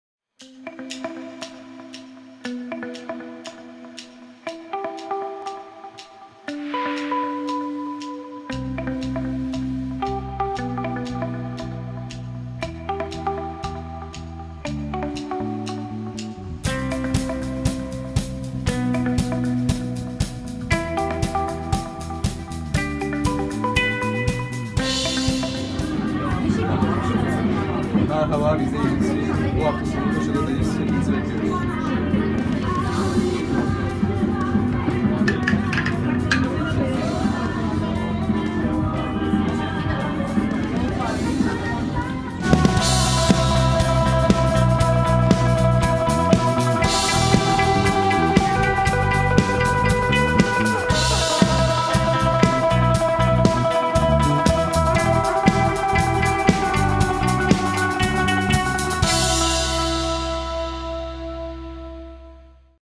Agency - Taşoda Teaser
We will be playing at Taşoda Festival in Boğaziçi University.The date is 2nd of May.